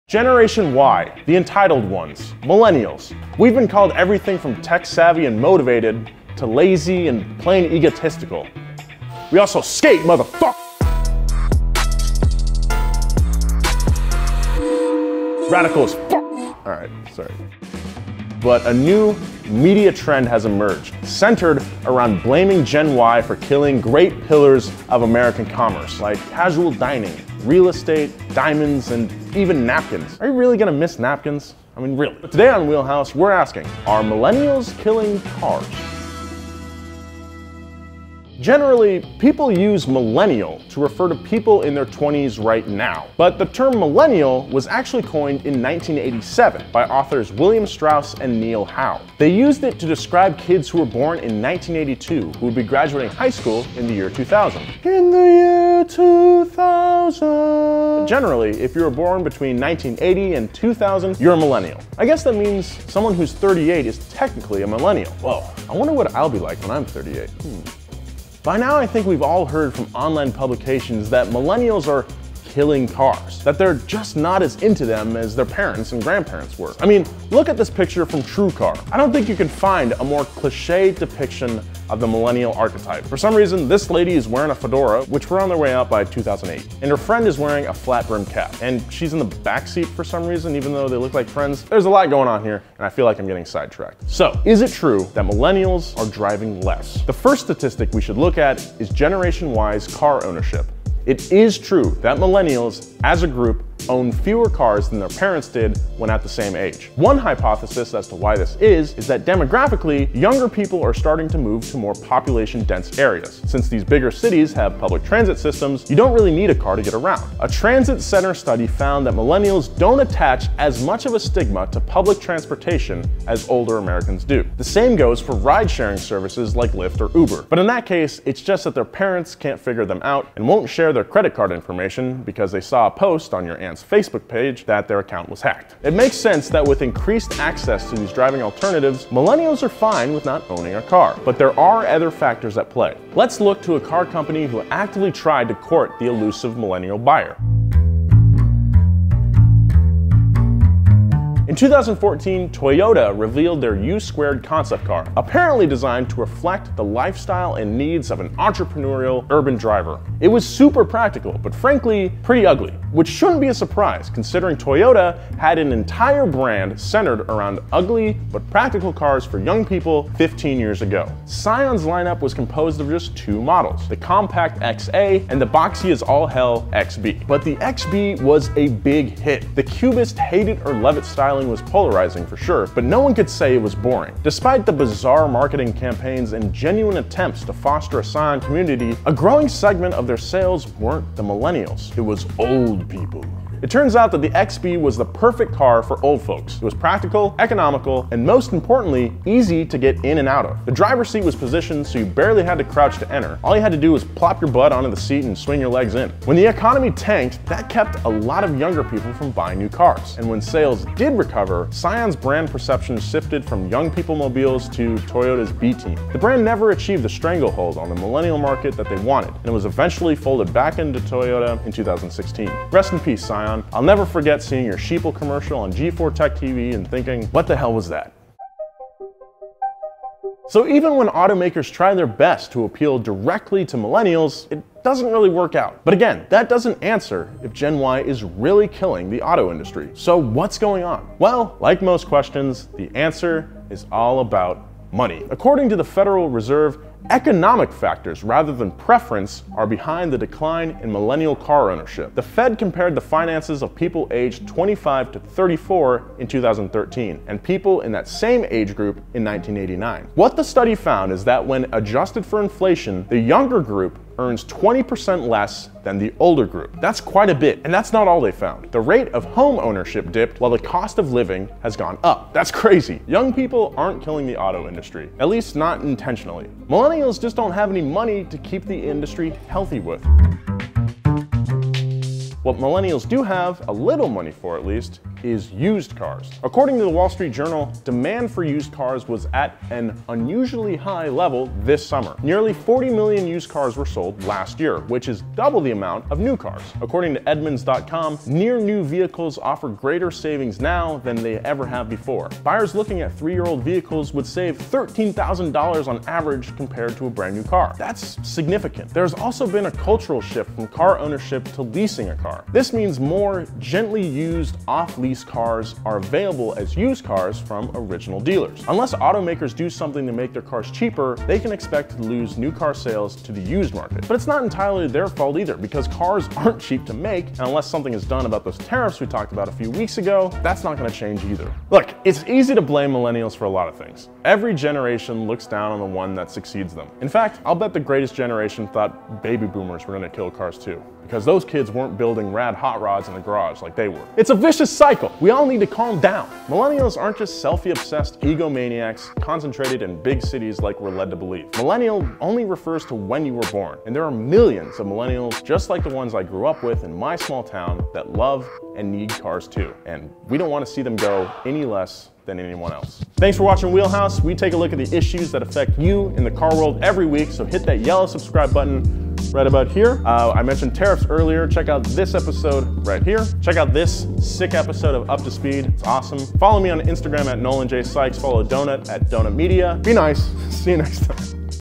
Are Millennials Killing Cars? | WheelHouse
We’ve all heard it before: Millennials are killing this, Millennials are killing that, They’re killing everything. But are they killing cars as a whole? Join Nolan as he digs for the truth and answers the question: Are millennials really killing cars?